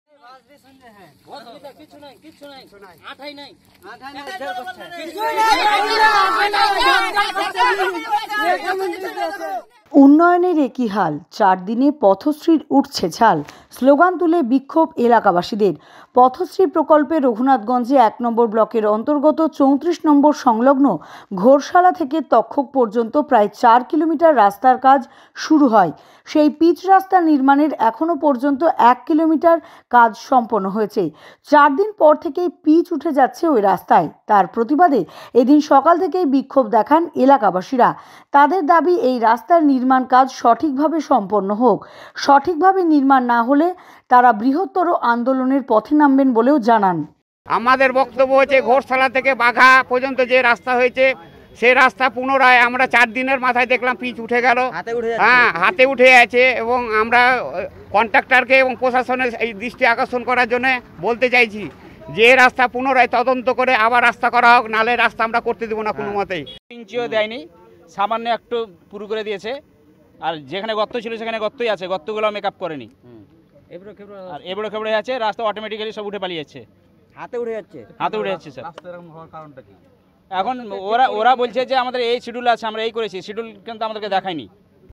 Pathashree: "উন্নয়নের একি হাল চারদিনে পথশ্রীর উঠছে ছাল" স্লোগান তুলে বিক্ষোভ এলাকাবাসীদের
তাদের দাবি এই রাস্তার নির্মাণ কাজ সঠিকভাবে সম্পন্ন হোক। সঠিকভাবে নির্মাণ না হলে আমরা আরও বৃহত্তর আন্দোলন করবো।